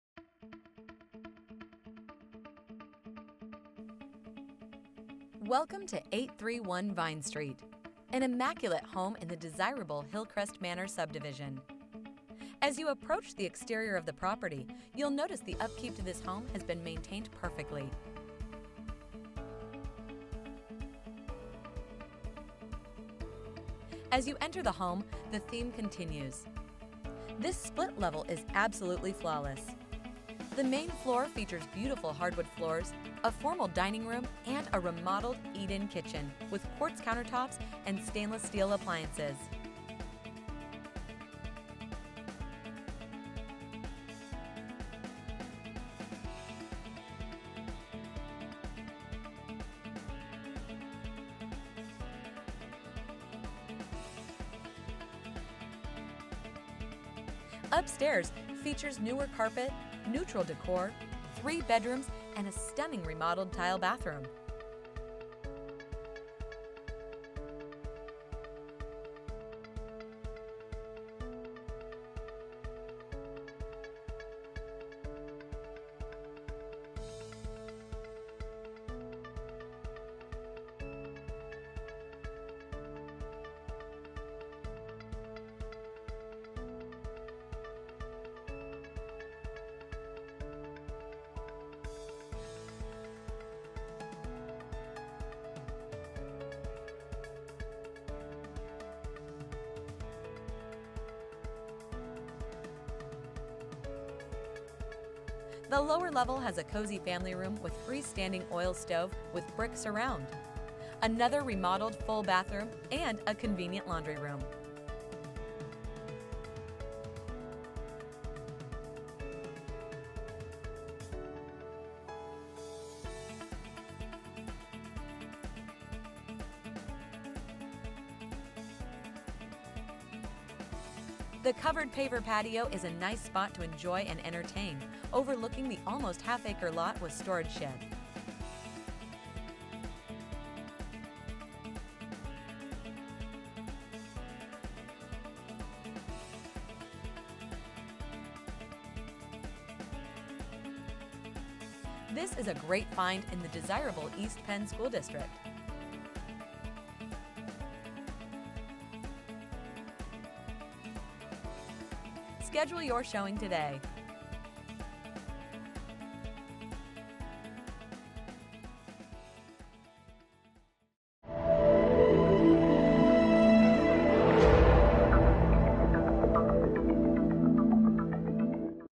831 Vine St. Macungie, PA 18062
Welcome to 831 Vine Street, an immaculate home in the
desirable Hillcrest Manor subdivision. As you approach the
exterior of the property, you’ll notice the upkeep to this
home has been maintained perfectly. As you enter the
home, the theme continues. This split-level is absolutely
flawless. The main floor features beautiful hardwood
floors, a formal dining room and a remodeled eat-in kitchen
with Quartz countertops and stainless steel appliances.
Upstairs features newer carpet, neutral décor, three
bedrooms, and a stunning remodeled tile bathroom. The
lower level has a cozy family room with freestanding oil
stove with brick surround, another remodeled full
bathroom, and a convenient laundry room. A partial
basement off the two-car garage offers lots of storage. The
covered paver patio is a nice spot to enjoy and entertain
overlooking the almost half-acre lot with storage shed. This
is a great find in the desirable East Penn school district.
Schedule your showing today.